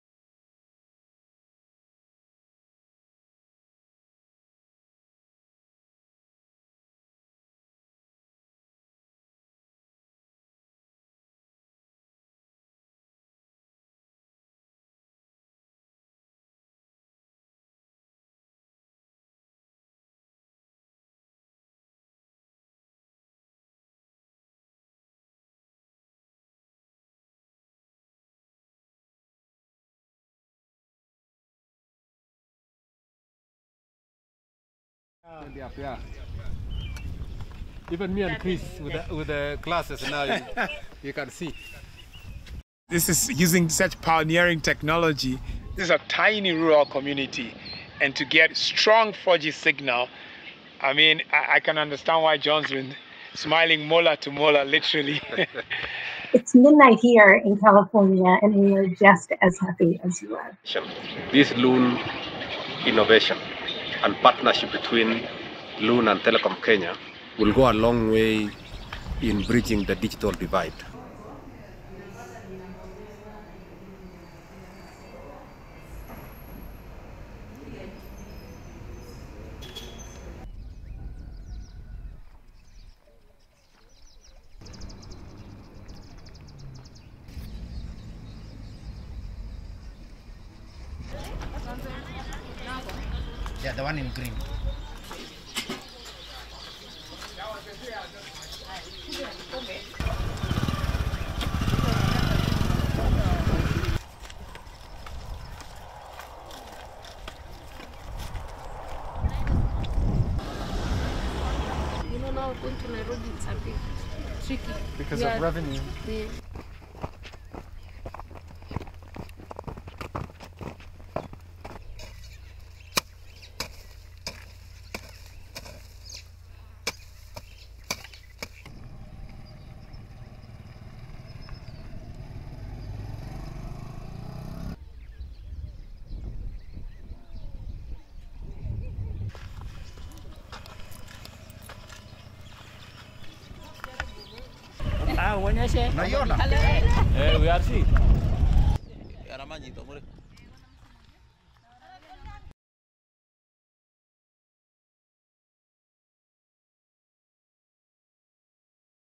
Telkom x Loon Connectivity Program
Telkom and Loon are proud to announce the availability of mobile Internet service via the Loon technology to Kenyans, starting July 2020. This important milestone comes as the project partners approach the completion of their network integration tests, that have seen them successfully assess service quality across their infrastructure and respective networks.